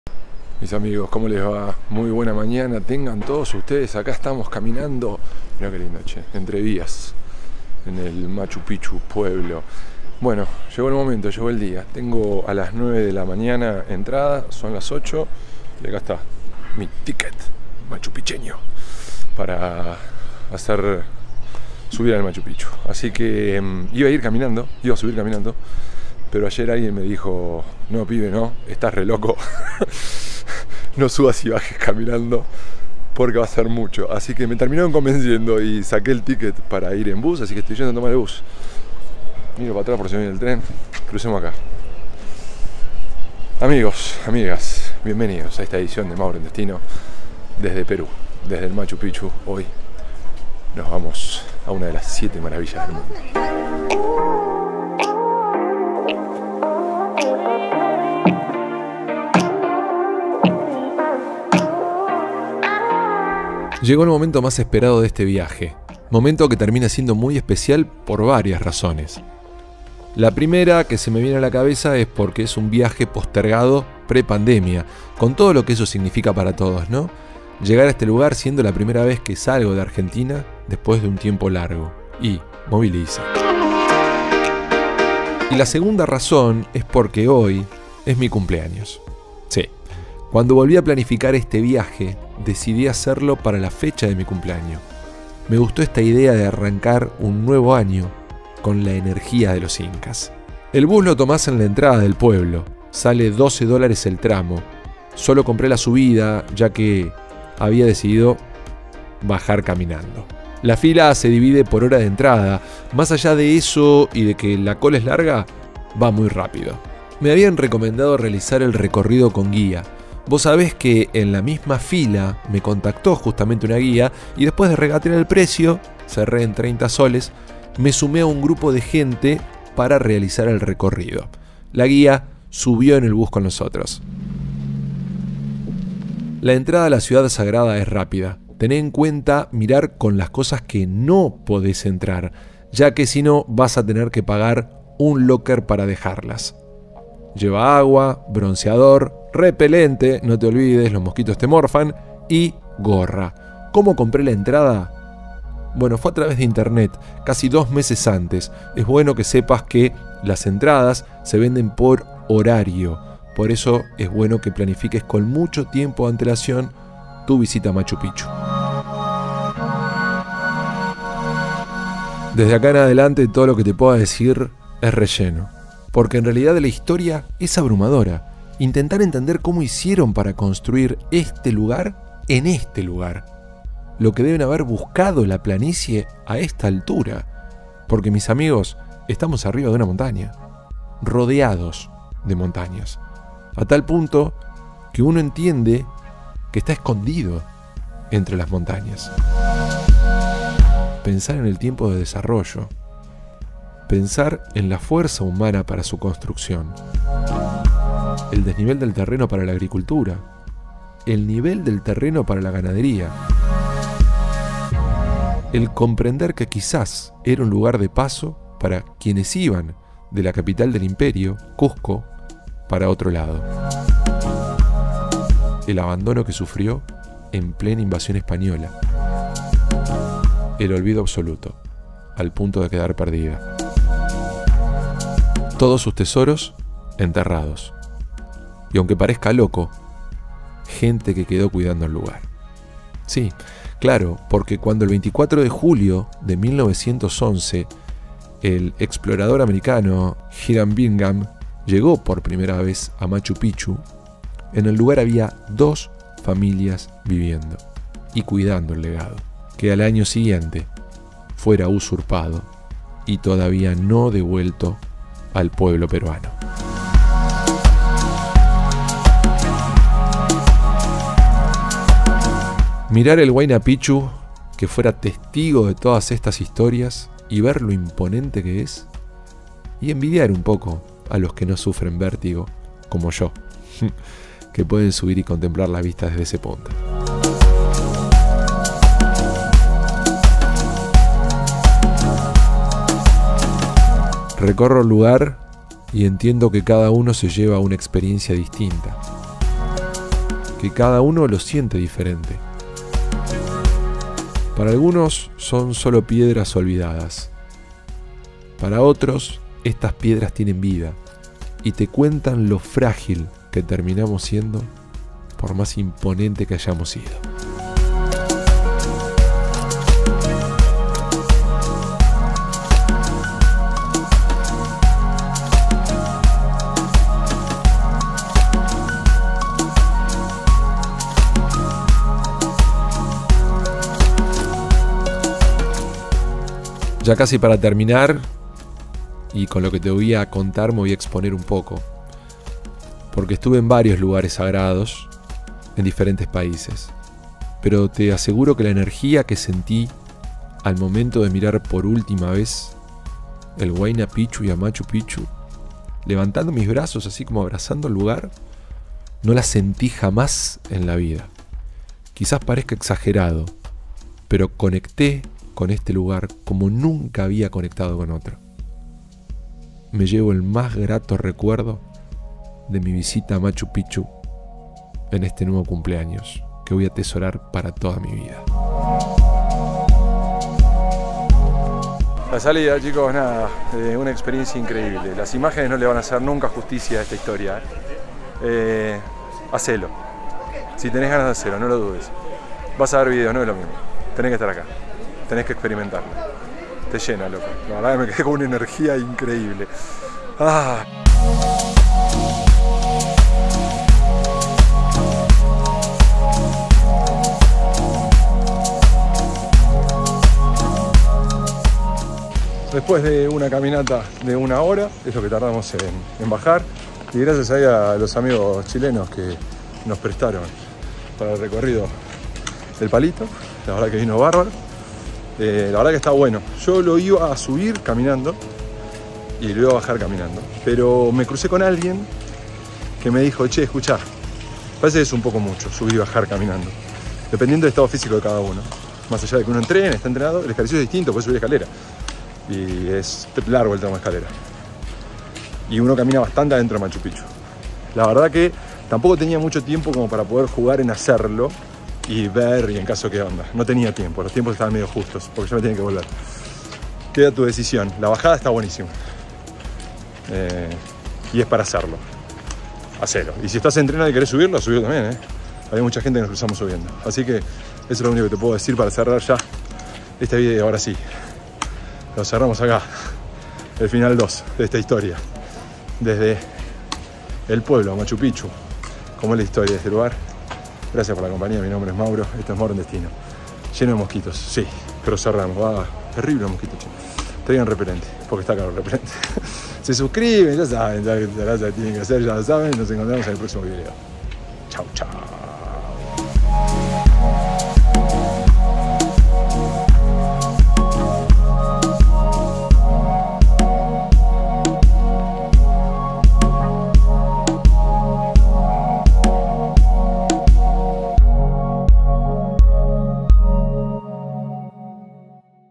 Una mañana en Machu Picchu - Peru - Mauro en Destino
En el video de hoy Mauro decide conocer Machu Picchu en una fecha muy especial para él.

Luego de mucho esperar este momento Mauro llega a la ciudad sagrada de los Incas para seguir conociendo la historia que nuestro mundo nos brinda.

Fecha de grabación: Octubre 2022